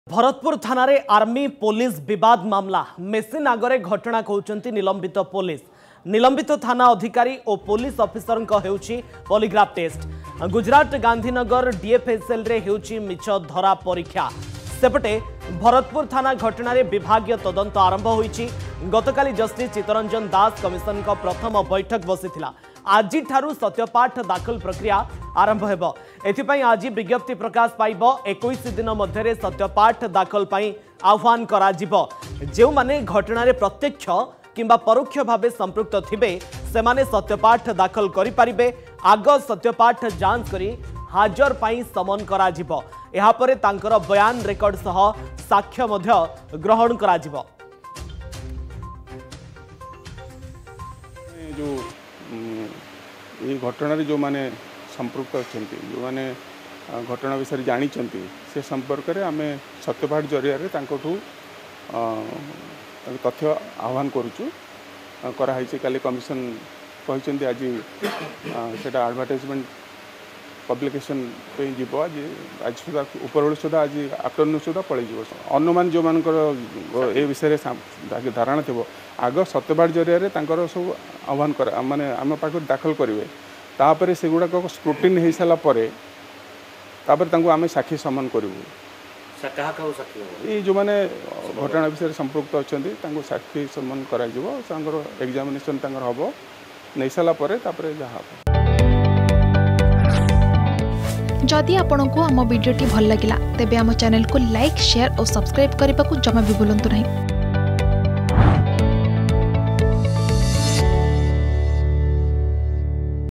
ଆଜିଠୁ ସତ୍ୟପାଠ ଦାଖଲ | Bharatpur Policestation | Argus News
ଭରତପୁର ଥାନାରେ ଆର୍ମି ପୋଲିସ ବିବାଦ ମାମଲା । ମେସିନ ଆଗରେ ଘଟଣା କହୁଛନ୍ତି ନିଲମ୍ବିତ ପୋଲିସ । ନିଲମ୍ବିତ ଥାନା ଅଧିକାରୀ ଓ ପୋଲିସ ଅଫିସରଙ୍କର ହେଉଛି ପଲିଗ୍ରାଫ ଟେଷ୍ଟ ।